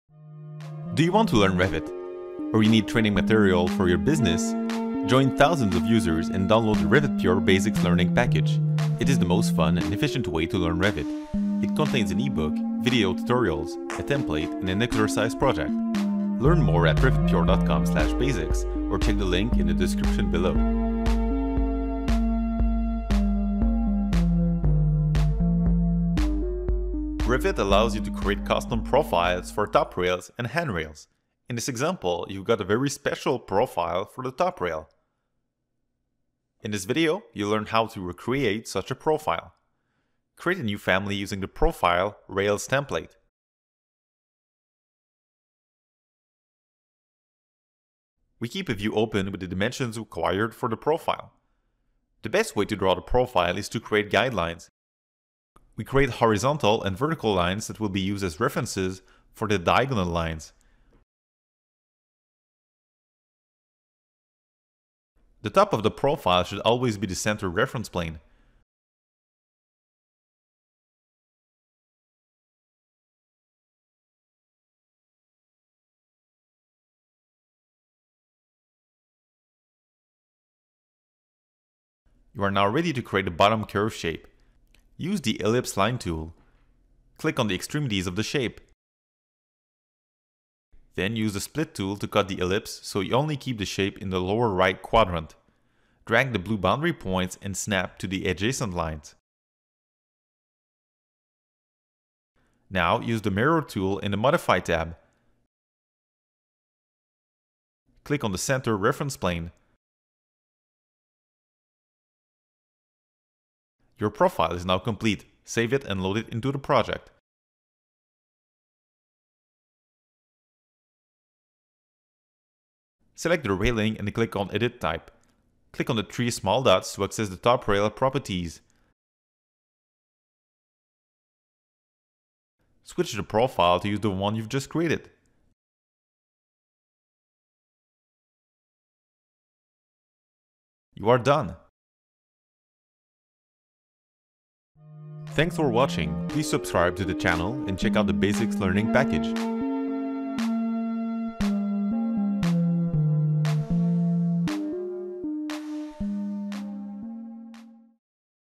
Revit Railings Masterclass - Custom Profile
Top rails and Handrails can be customized to use profiles created by users. In this tutorial, we modify a top rail to use a strange profile.